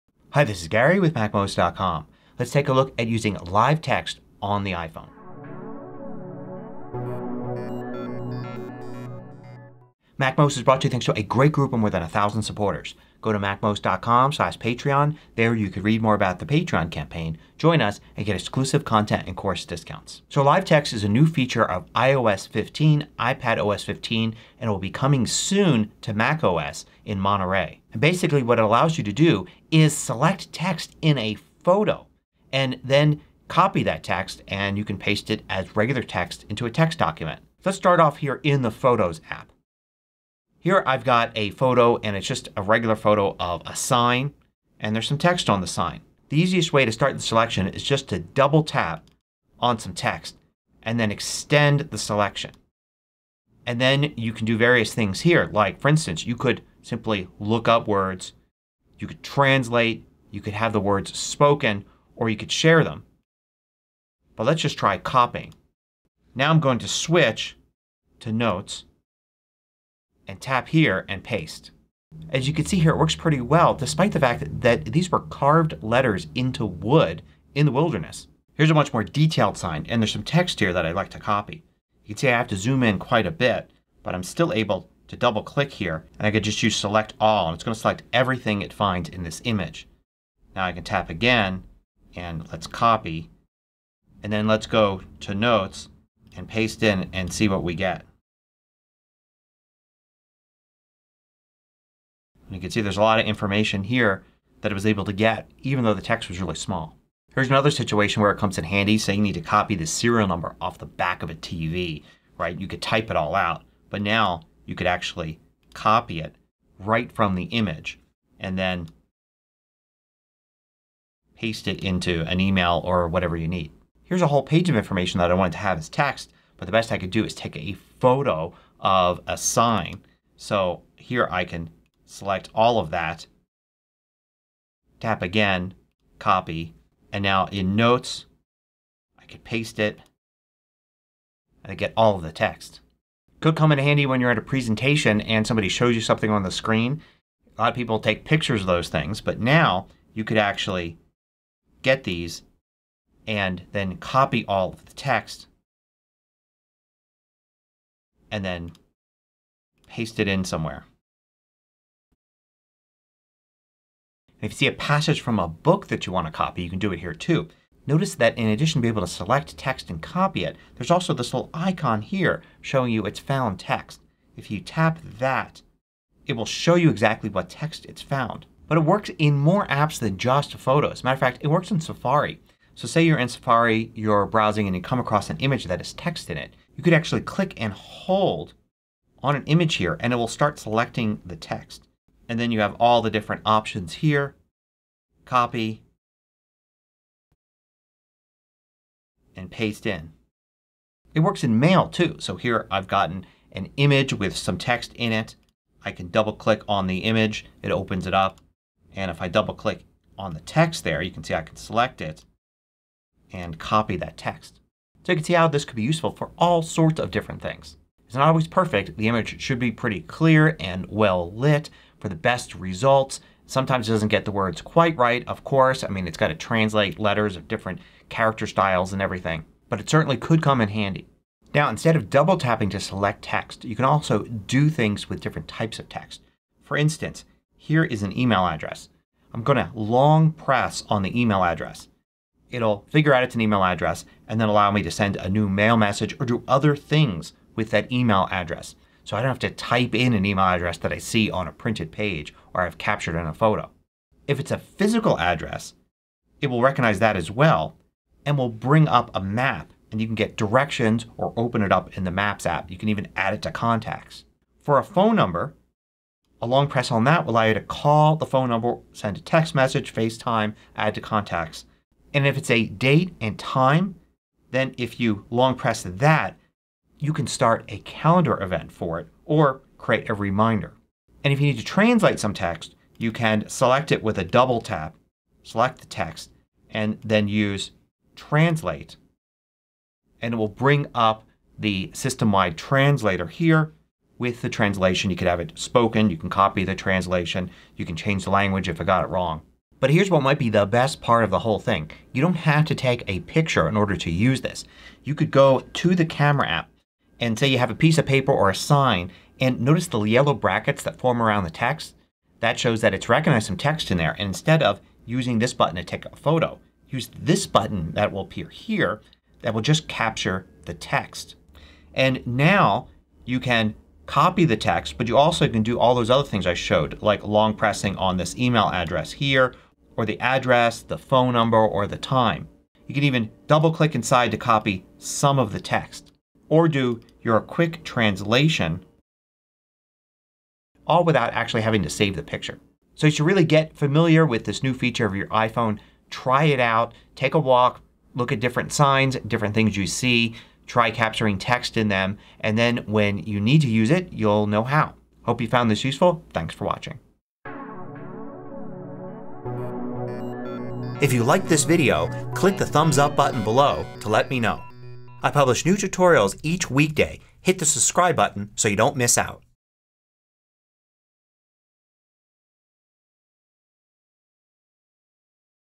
Using the Amazing New Live Text Feature On Your iPhone
Live Text allows you to copy text in your photos and take other actions as well. You can use it on your existing photos and you can also capture text without taking a photo at all.